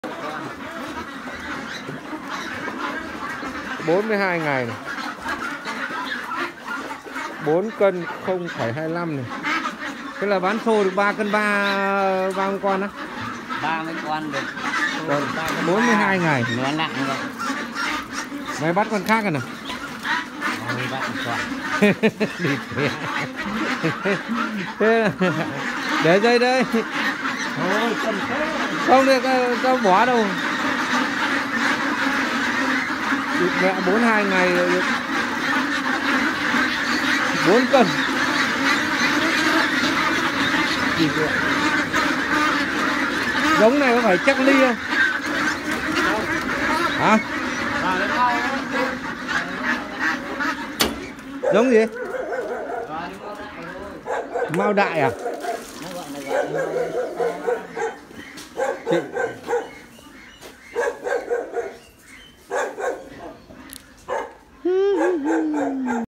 Trại Vịt thịt quy mô to 5 ngàn đang sử dụng X322 super của Phúc Vlog. 42 ngày tuổi quý anh chị em ah
Trại vịt thịt Quy mô to lớn, sử dụng cám X322Super quá đẳng cấp!
42 ngày tuổi. Co to 4kg
Phúc cám luôn sẵn sàng phục vụ quý khách hàng trải nghiệm và sử dụng cám chất lượng nhất, giá hợp lý nhất.
Cám ơn mọi người nhiều nhé